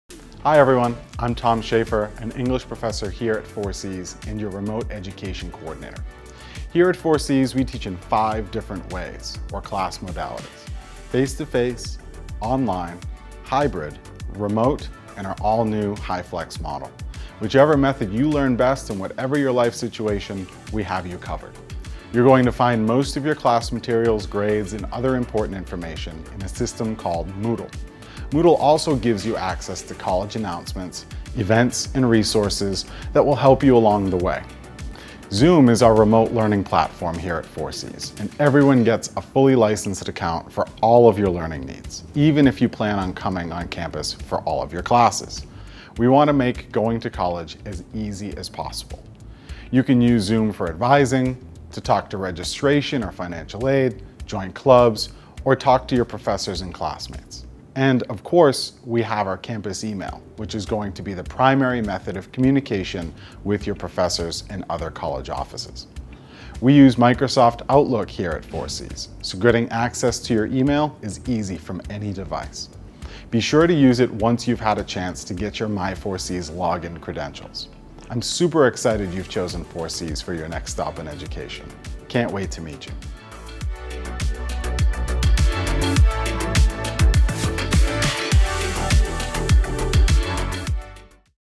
4Cs Orientation - Your Classes and Online Tools
Tom Schaeffer, 4Cs English Professor and Remote Education Coordinator, takes you through the different types of technology available to you to succeed in the classroom. From Zoom to Outlook and Microsoft Word, Tom has the answers for what you'll need.